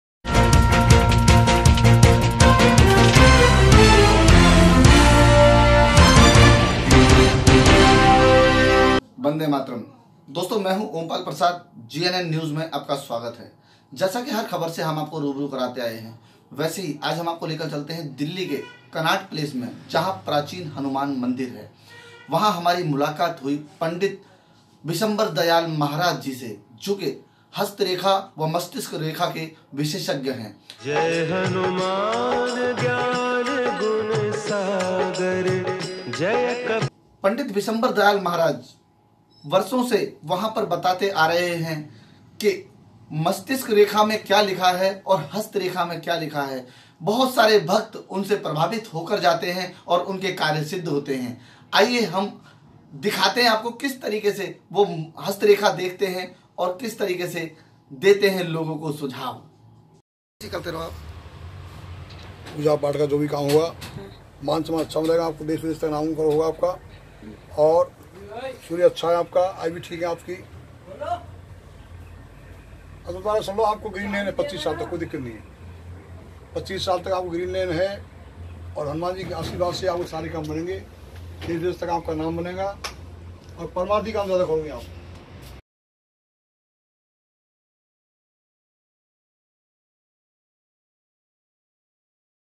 GNN NEWS : Chamatkar Dikha Rahe Pandit Vishambhar Dayal Maharaj Hanuman Mandir CP Delhi
GOODWILL MEDIA HOUSE
GNN NEWS AGENCY
GNN NEWS INDIA